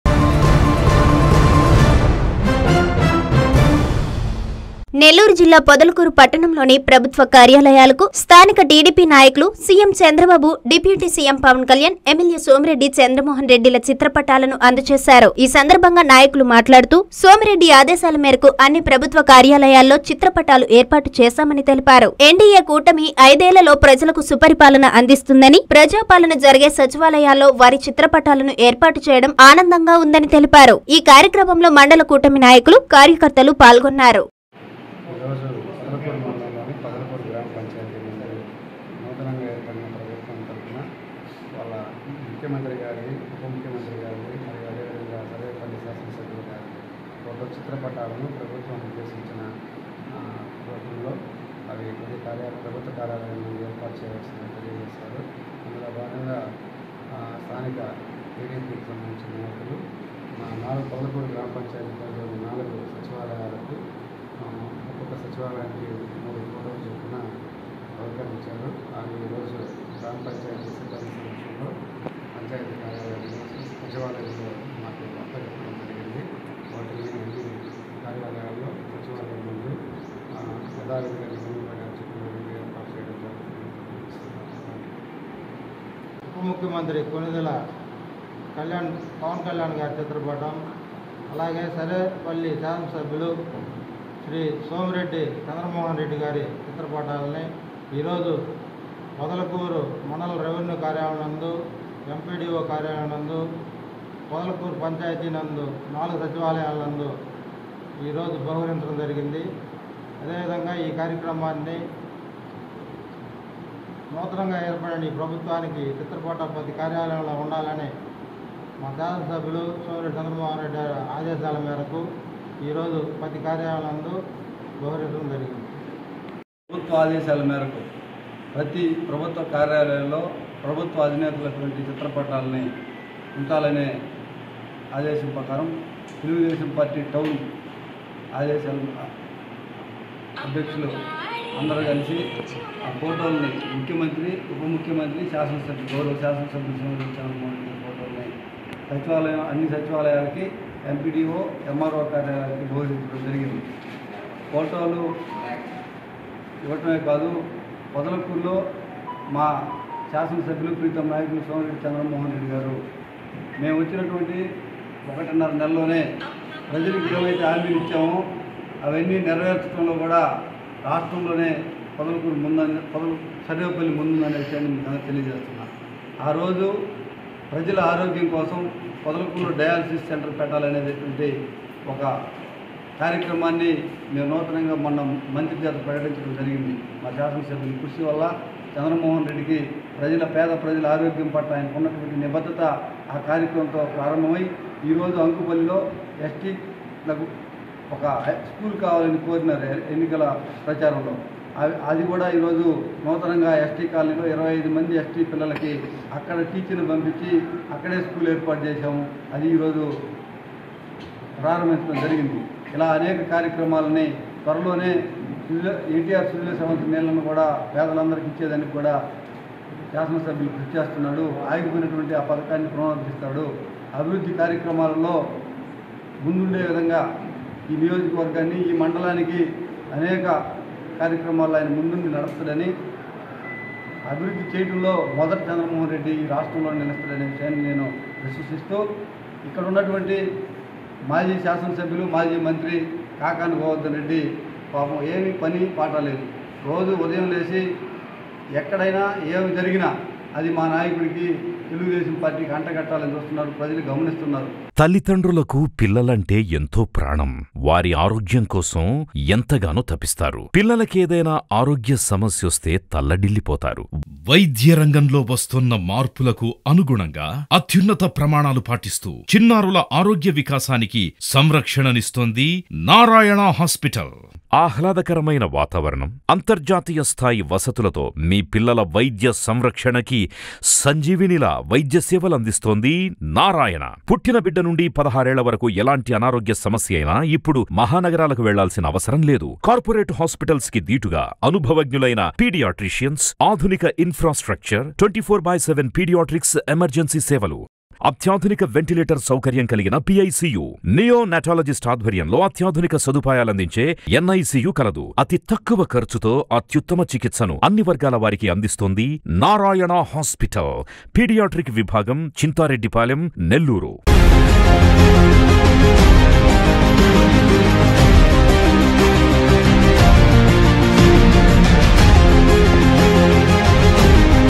ఎన్టీఏ కూట‌మిలోనే ప్ర‌జ‌ల‌కు సుప‌రిపాల‌న‌@N3NEWS
- పొద‌ల‌కూరు ప్రభుత్వ కార్యాలయాల్లో సీఎం, డిప్యూటీ సీఎం, ఎమ్మెల్యే చిత్రపటాలు బహుకరణ